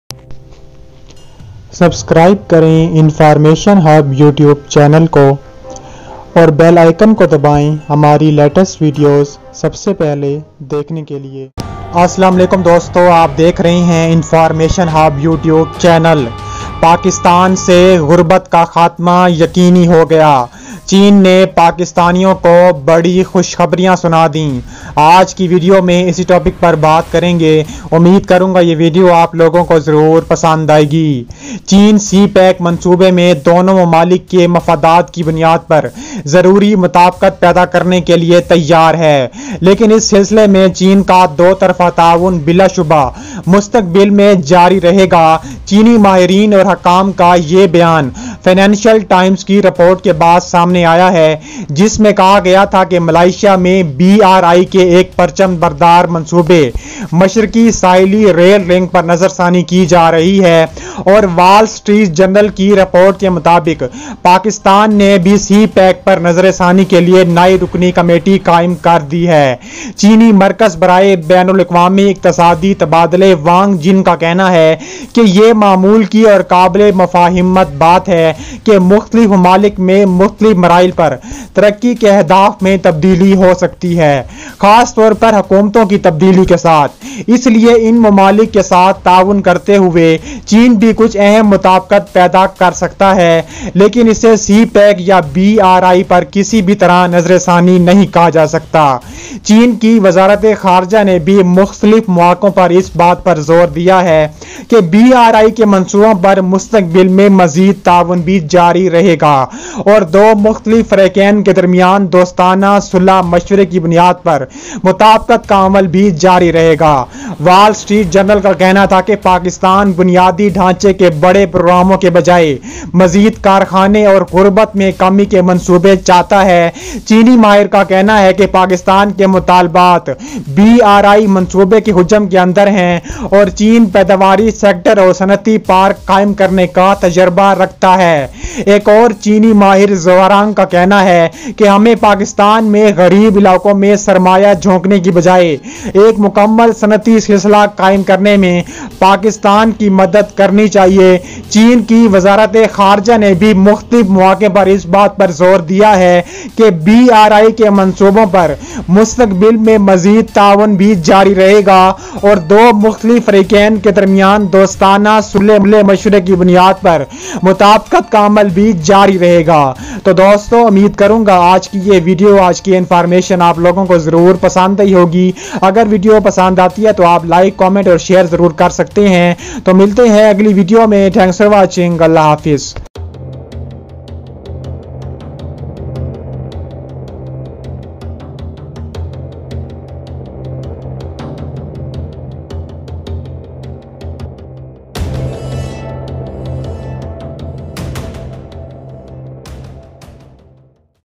China Pakistan New Devolpment September 2018
Assalam-o-Alaikum Doston,
Doston Aj Ki Video Main Aapko Batao Ga K China ny Pakistan Sy Ghurbat Khtm Krny ka Bara Elan kr diya.. Agr Video Pasand Aaye To Like Aur Share Zror Kijye Ga and Thanks for Your Time and Support..!!!